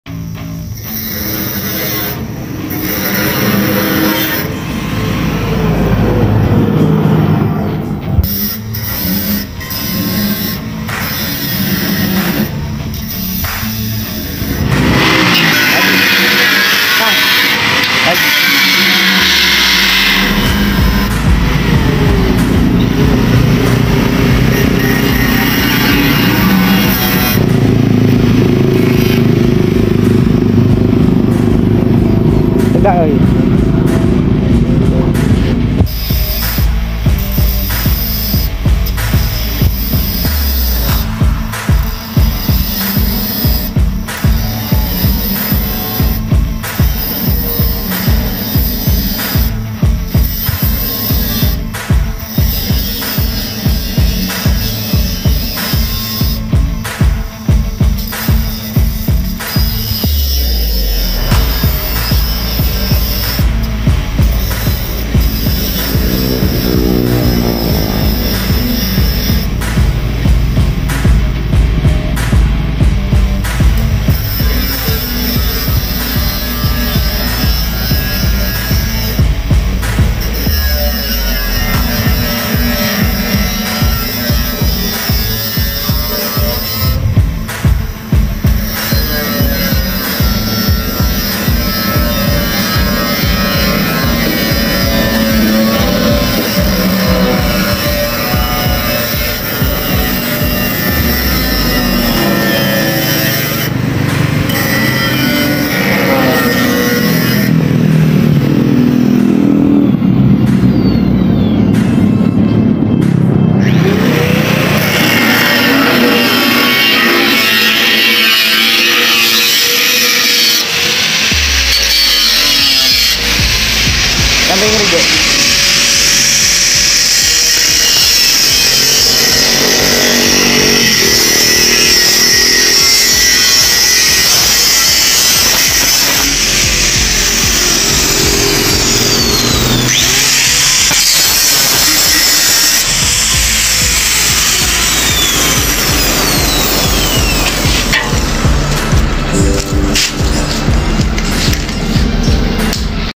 Cara Aman Bongkar Tanki Motor
Vidio ini menunjukan Cara Bongkar Tanki Motor dengan gerinda,atau las,
Cara Bongkar Tanki Motor yg pertama harus di cuci dulu dalam tanki dg air rinso,kemudian di kuran sampai hilang bau bensin,Cara Bongkar Tanki Motor ini akn lebih aman karena sangat berbahaya jika Cara Bongkar Tanki Motor sembarangan akan bisa menyebabkan ledakan yg berbahaya